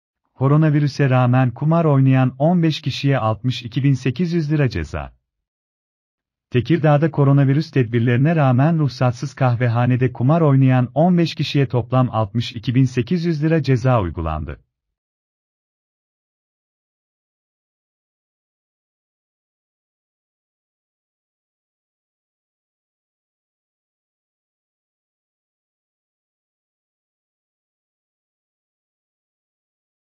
Koronavirüse rağmen kumar oynayan 15 kişiye 62 bin 800 lira ceza
Tekirdağ’da koronavirüs tedbirlerine rağmen ruhsatsız kahvehanede kumar oynayan 15 kişiye toplam 62 bin 800 lira ceza uygulandı.